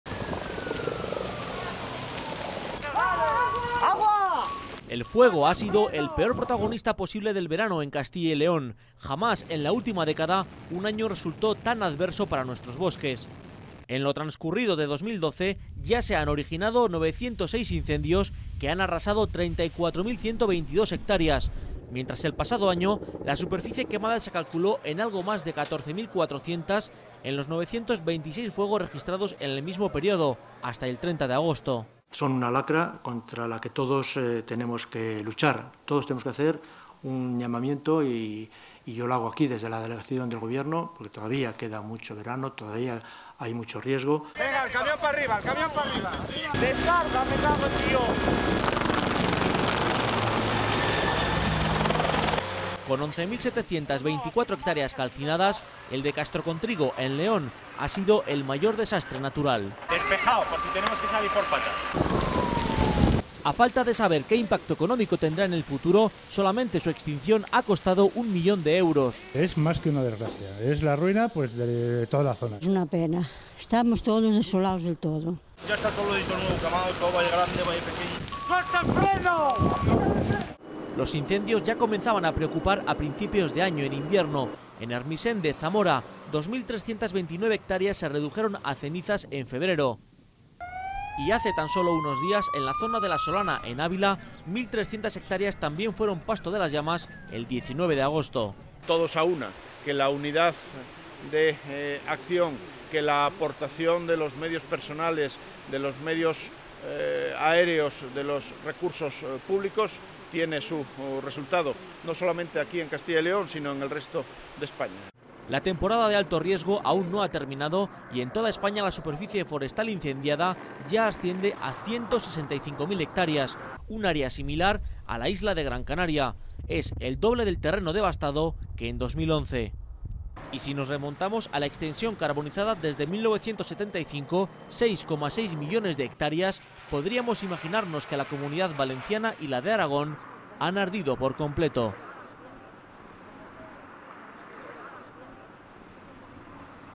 Balance de los incendios para Espacio Abierto
El fuego ha sido el peor protagonista posible del verano en Castilla y León. Jamás en la última década un año resultó tan adverso para nuestros bosques. En lo transcurrido de 2012 ya se han originado 906 incendios que han arrasado 34.122 hectáreas. Mientras, el pasado año la superficie quemada se calculó en algo más de 14.400 hectáreas en los 926 fuegos registrados en el mismo periodo, hasta el 30 de agosto.